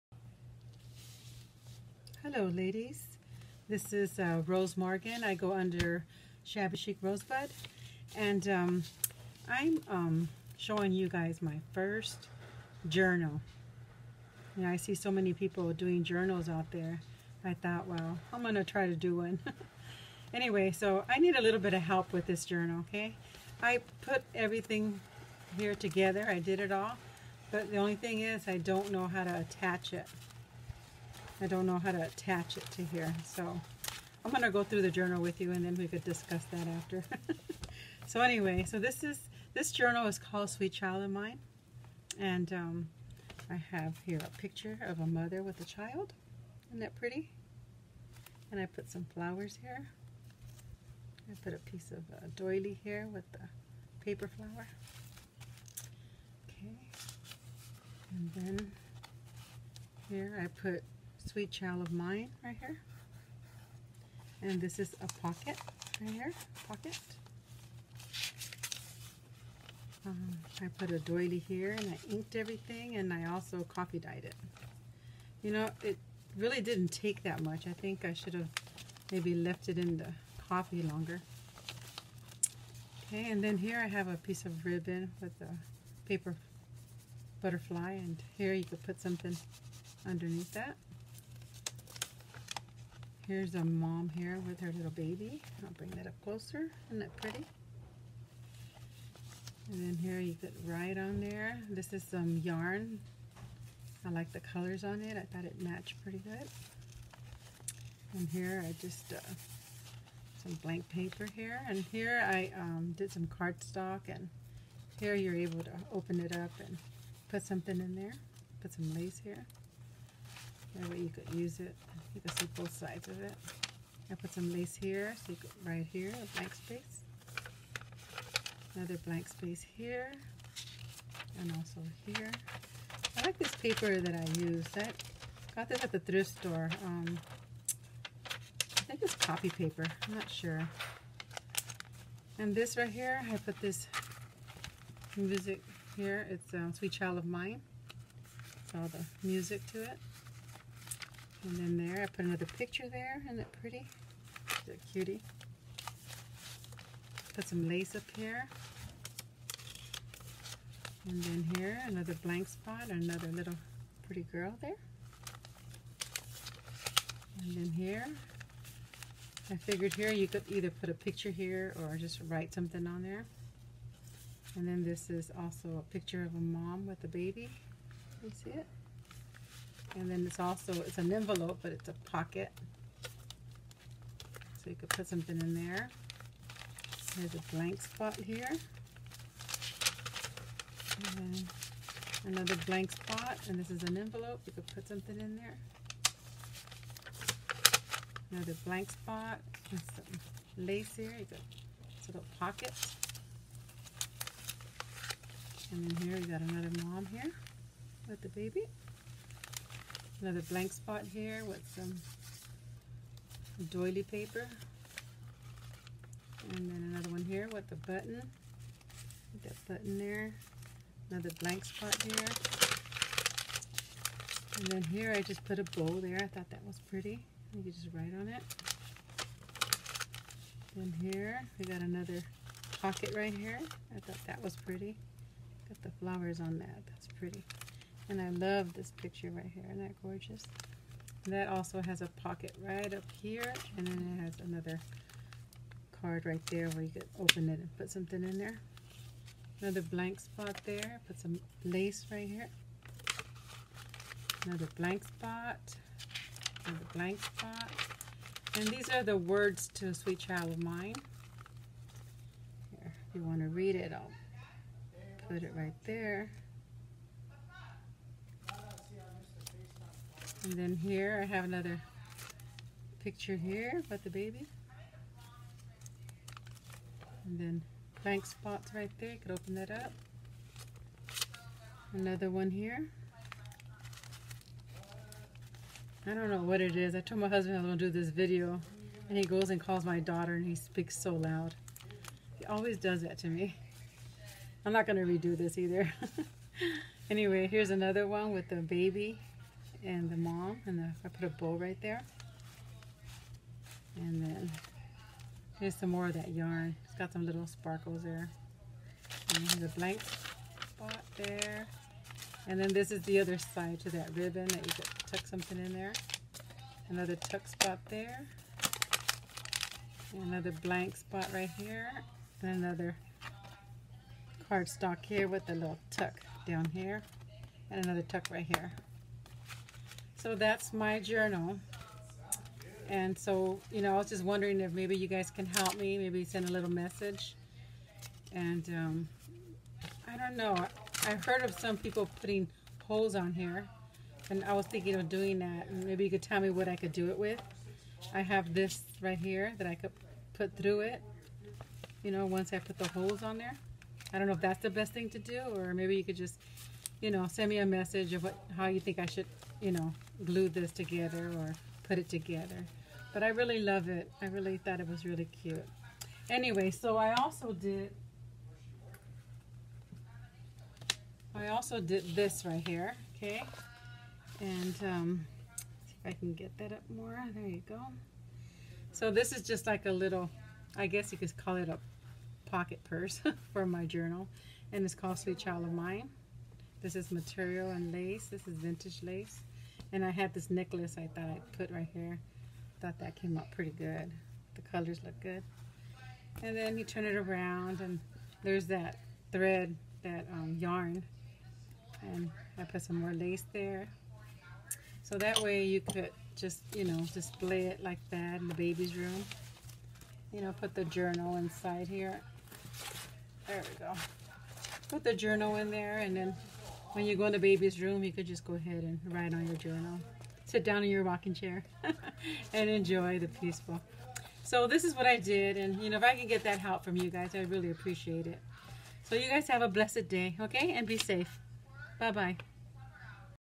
Shabby chic Rosebud ...My first journal (not for kids)
This is my first journal and I named it Sweet Child of mine...I didn’t finish it because I didn’t know how to put it together I was hoping maybe I can get some help from you and maybe you can message me on how to put it or what’s  the best way to put it together...❤️💝💝